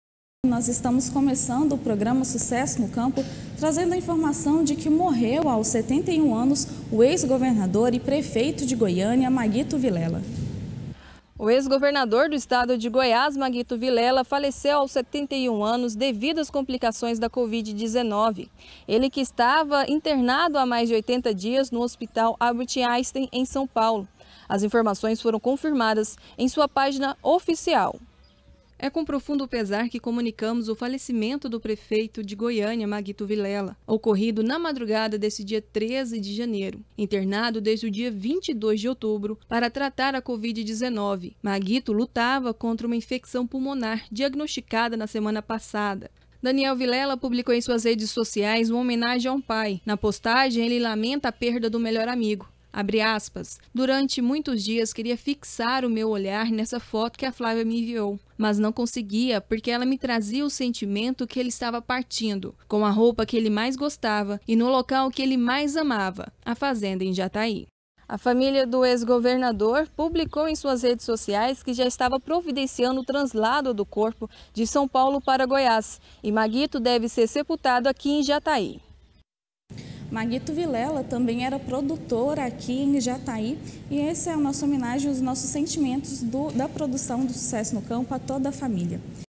Morre aos 71 anos, o prefeito de Goiânia Maguito Vilela
O ex-governador Maguito Vilela (MDB), faleceu devido às complicações da Covid-19, após ficar 81 dias, internado no hospital Albert Einstein, em São Paulo.